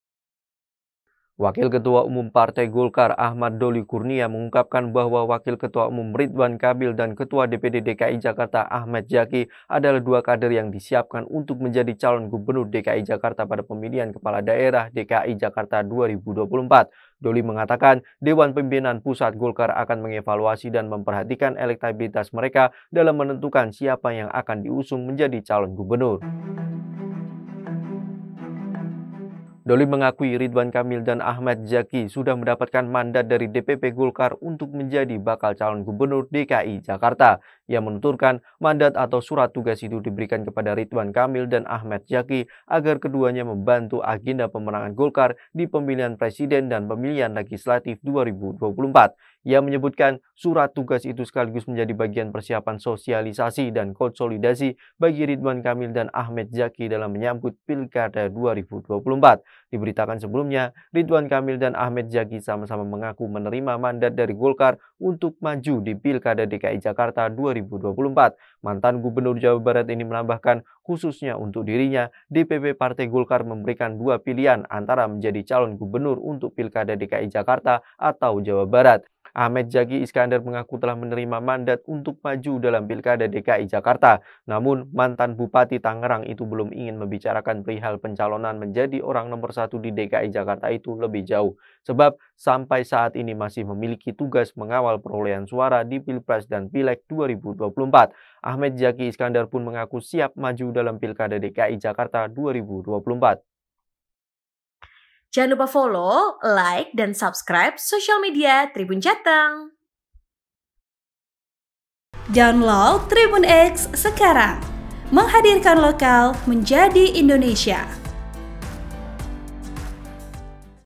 Golkar Beri Mandat Ridwan Kamil Bertarung di Pilgub DKI
Wakil Ketua Umum Partai Golkar Ahmad Doli Kurnia mengungkapkan bahwa Wakil Ketua Umum Ridwan Kamil dan Ketua DPD DKI Jakarta Ahmed Zaki adalah dua kader yang disiapkan untuk menjadi calon gubernur DKI Jakarta pada pemilihan kepala daerah (Pilkada) DKI Jakarta 2024.

Doli menyatakan, Dewan Pimpinan Pusat (DPP) Golkar akan mengevaluasi dan memperhatikan elektabilitas mereka dalam menentukan siapa yang akan diusung menjadi calon gubernur.